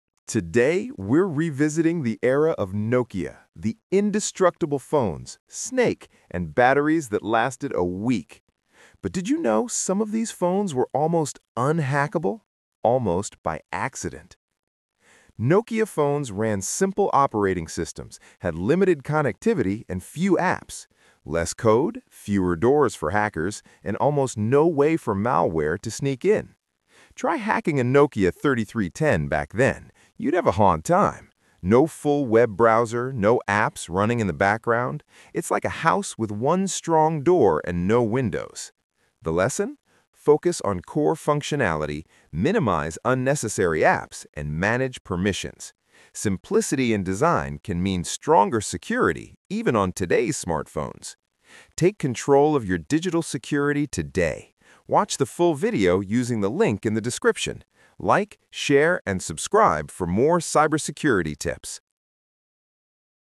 How Nokia Accidentally Created The Most Secure Phone Ever Made.
Old Nokia phones were nearly unhackable — and not because they were advanced.

📞🛡️ In this video, we revisit classic Nokia devices to explain how simplicity, limited connectivity, and fewer apps accidentally made them incredibly secure.

You’ll see why less code meant fewer vulnerabilities, how modern smartphones introduced massive attack surfaces, and the key security lesson we can still apply today: reduce what you don’t need, manage permissions, and keep things simple.

If you’re interested in phone security, tech history, or how old design choices can teach modern cybersecurity lessons, this quick breakdown delivers the insight fast.